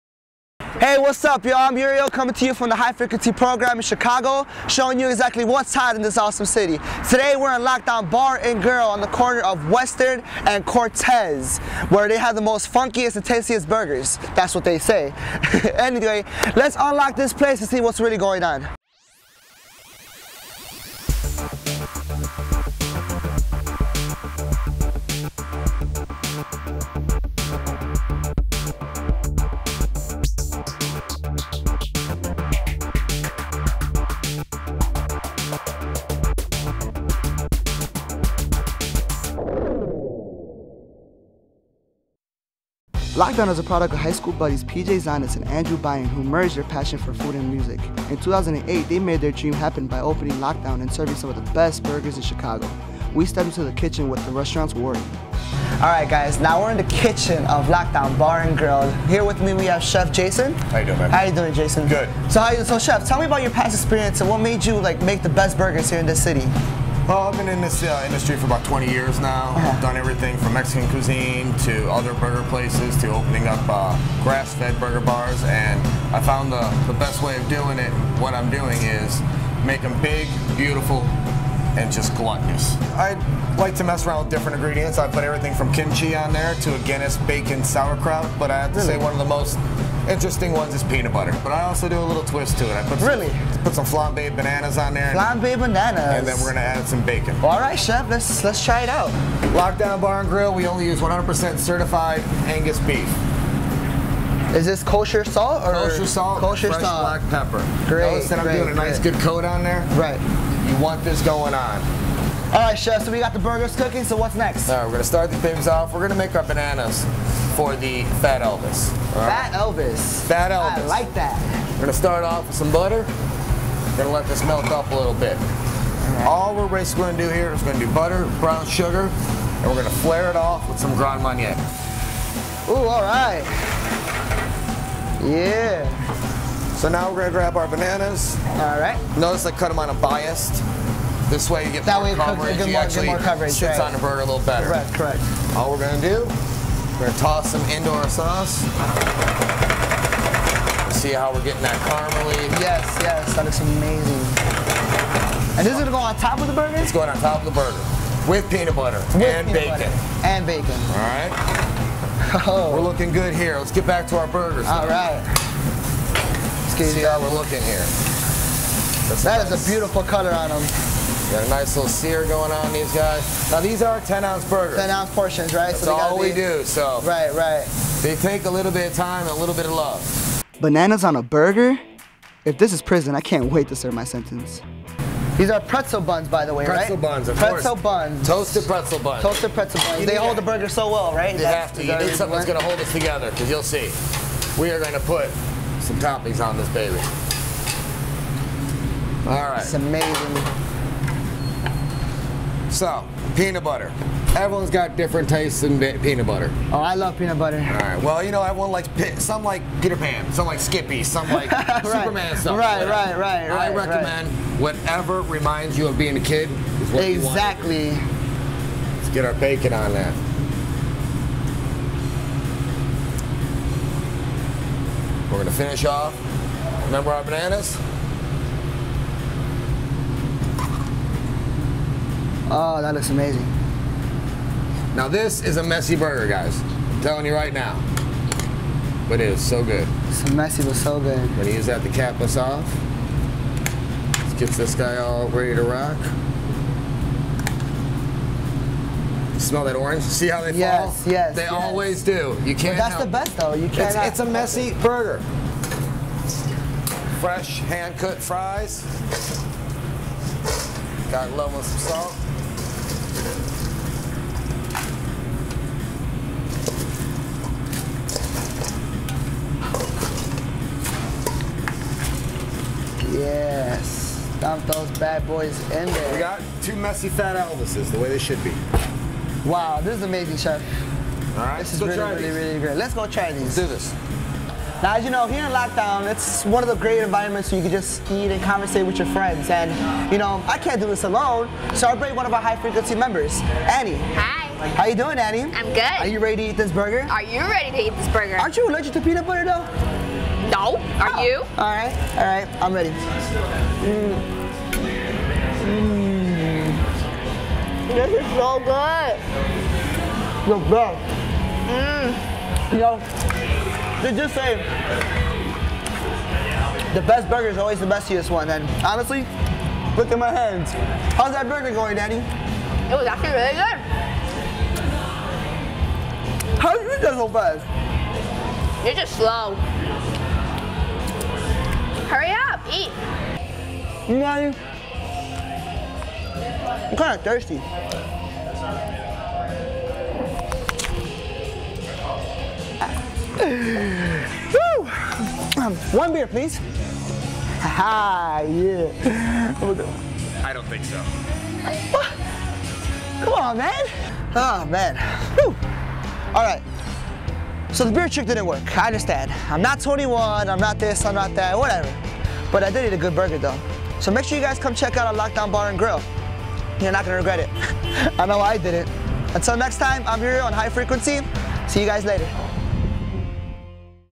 Lockdown Bar and Grill
The Hi Frequency crew set out to find Chicago's tastiest burger.  That journey led them to Lockdown Bar & Grill in Ukrainian Village, where the burgers are known for their outrageous  names and unforgettable flavors.